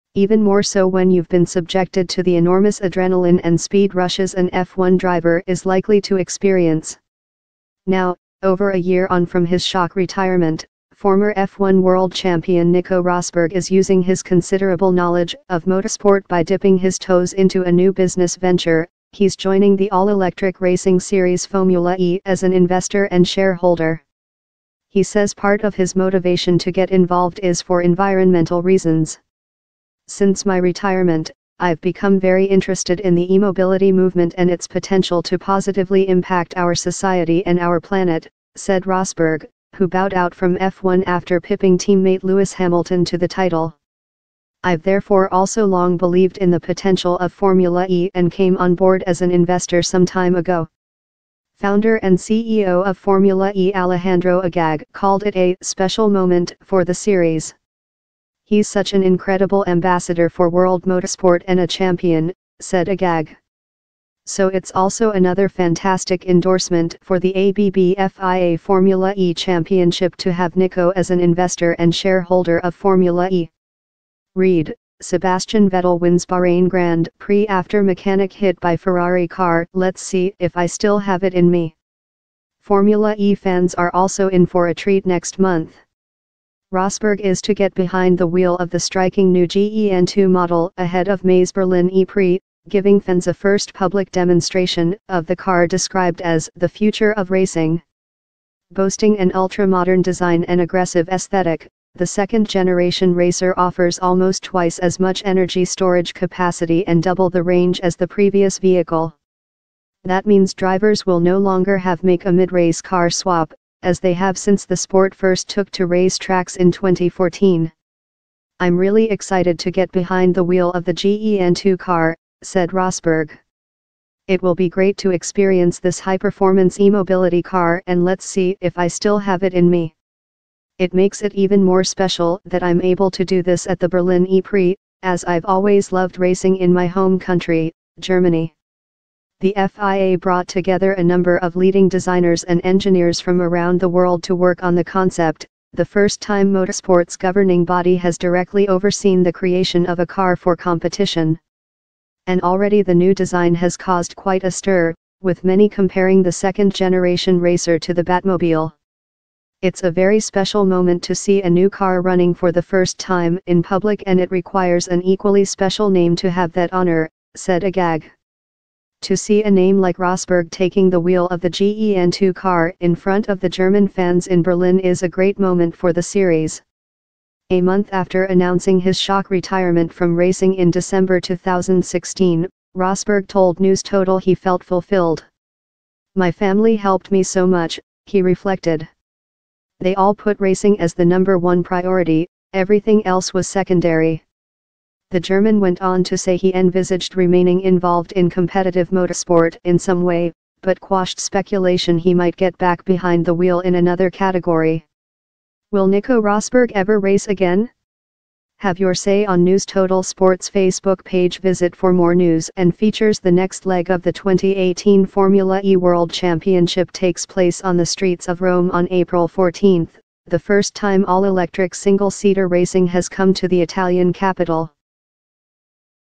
Nico Rosberg: Former F1 World Champion Becomes Formula E Investor
Even more so when youve been subjected to the enormous adrenaline and speed rushes an F1 driver is likely to experience.